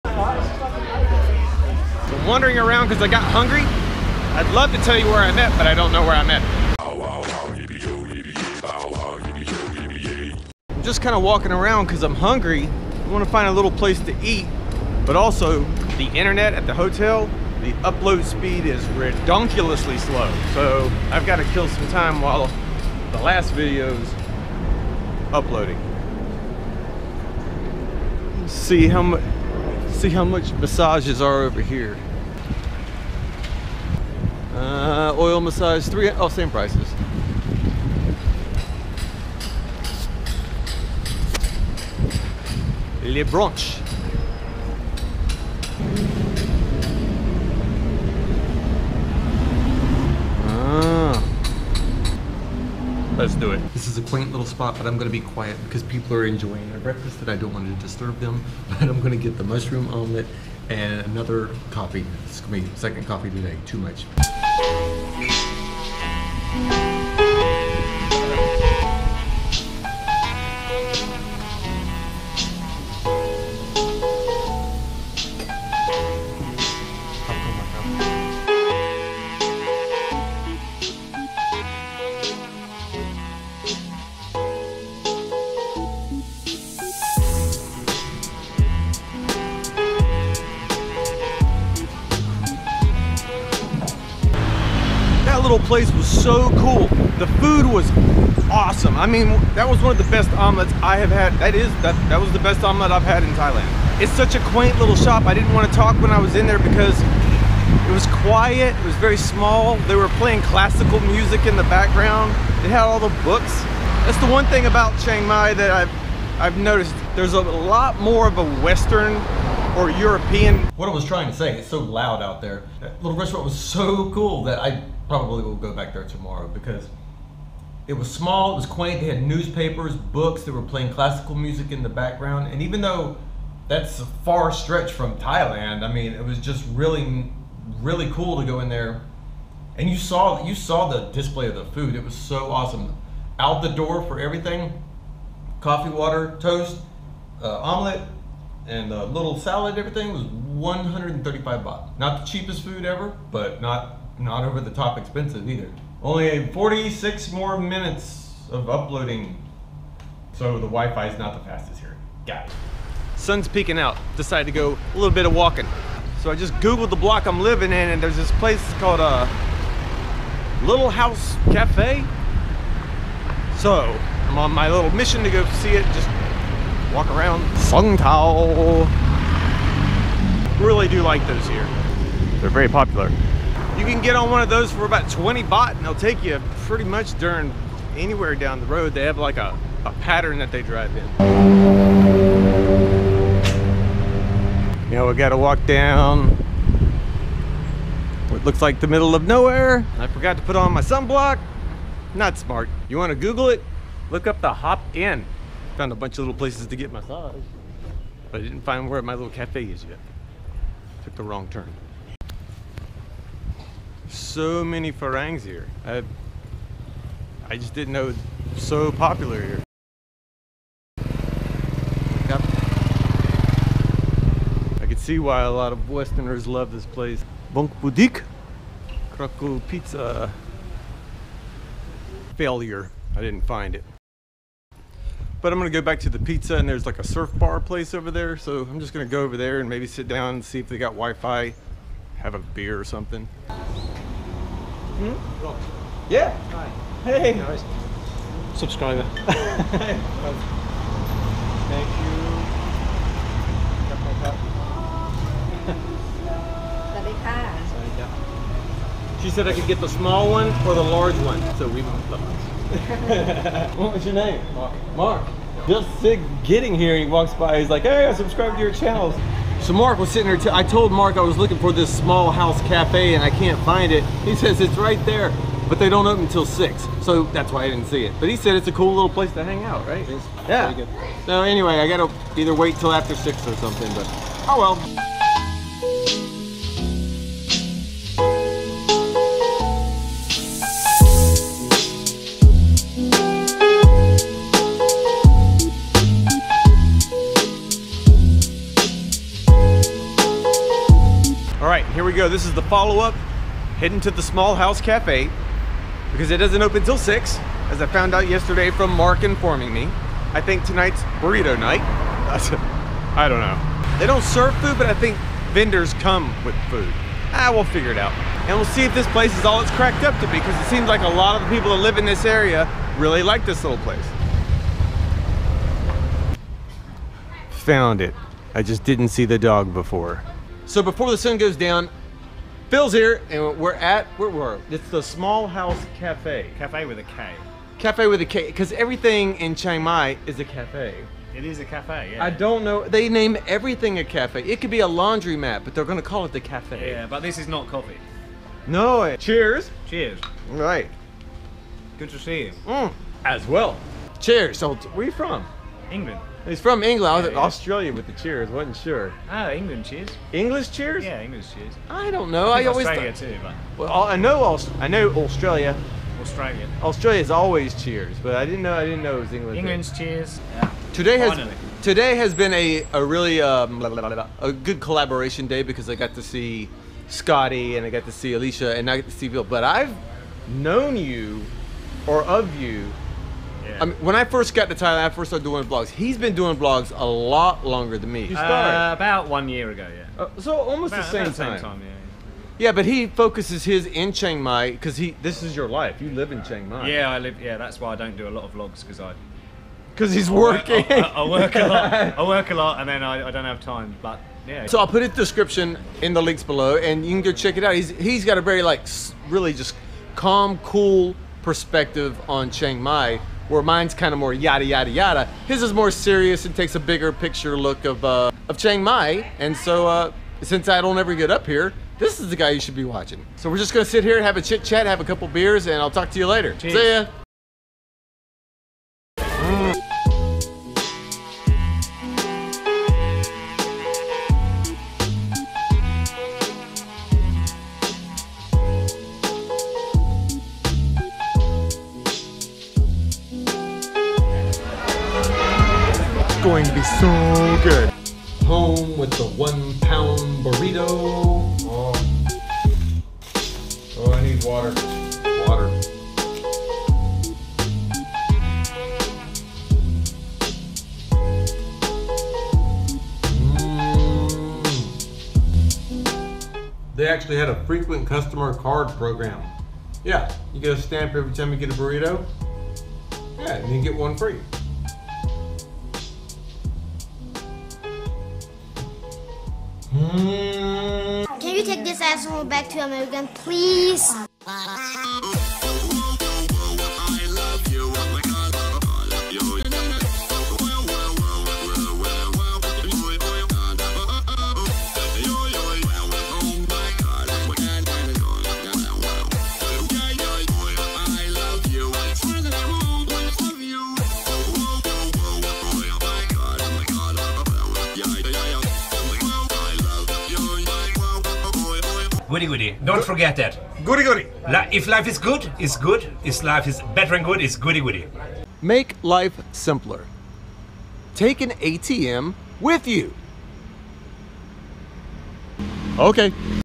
BURRITO NIGHT IN CHIANG MAI V171
I went looking for food in Chiang Mai and was surprised by what I found.

Shipping Address.
The address to ship to is:
Thailand Rob
C/O White Sun Bar
121 Moo 3, Beach Rd